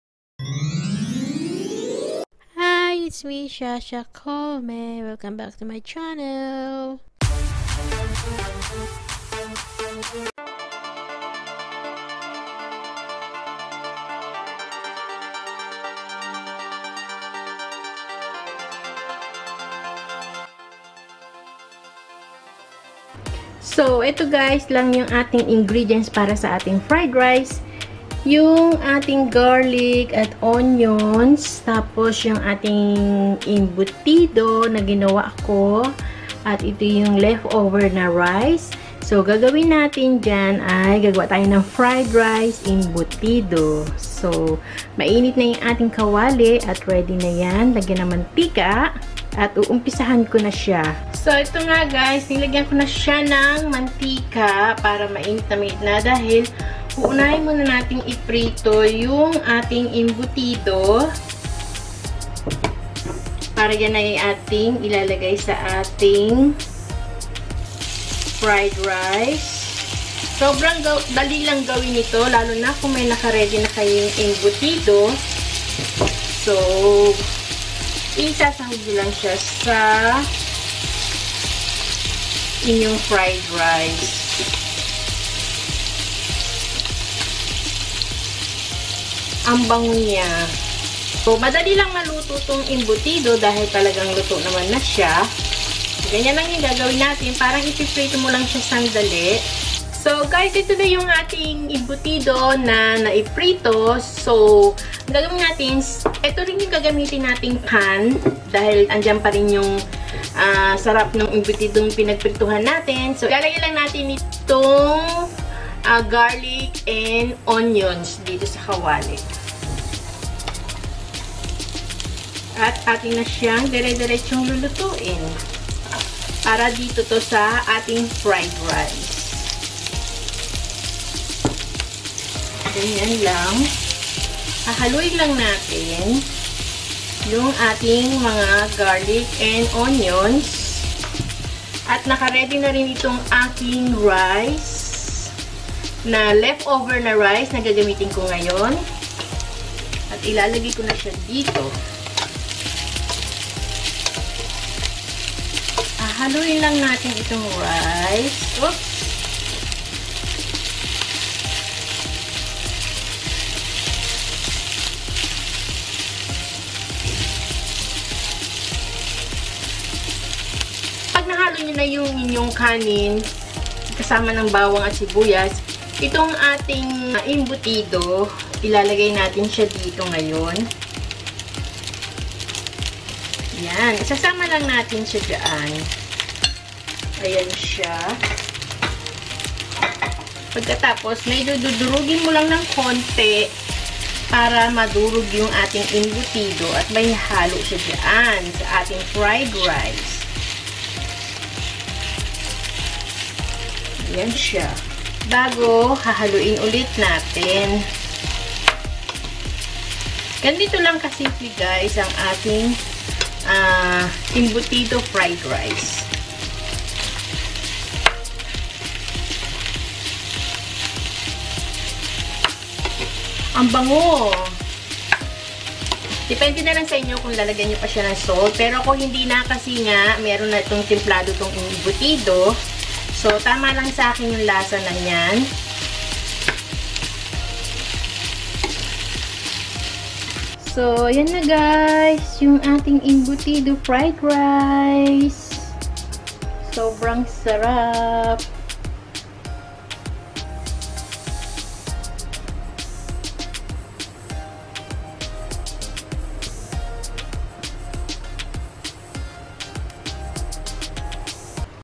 How To Cook Embutido Fried Rice | Embutido Fried Rice Recipe | Pork Embutido| ShaSha Kolme Vlogs
Embutido Fried Rice it's a Filipino Breakfast Style quick and easy.

shasha kolme